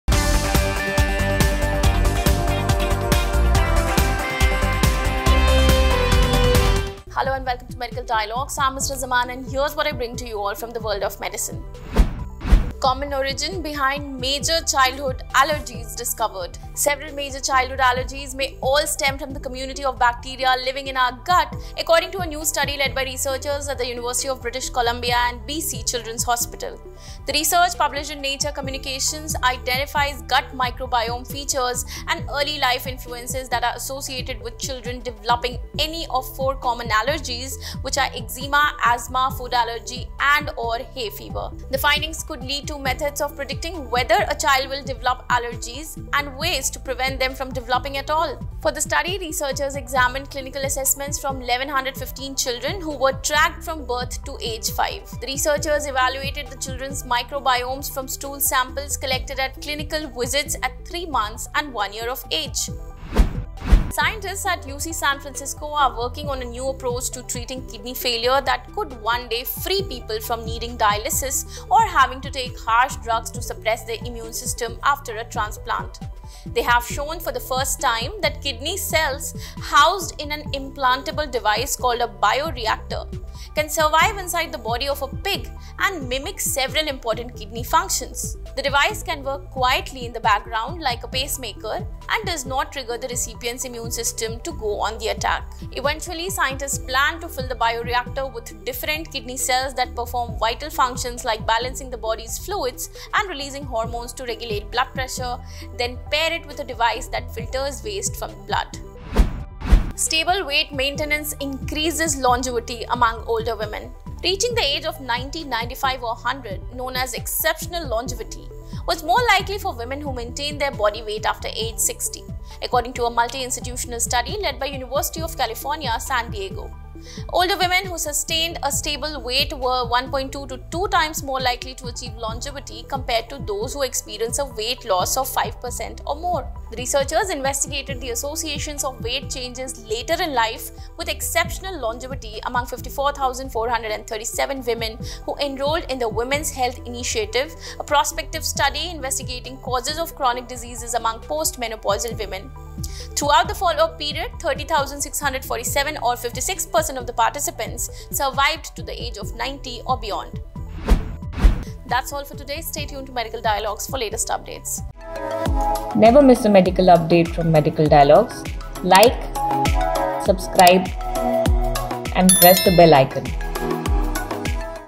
TOP MEDICAL NEWS 30.8.23
1. Common origin behind major childhood allergies discovered
2. Artificial kidney may free patients from dialysis
3. Stable weight maintenance increases longevity among older women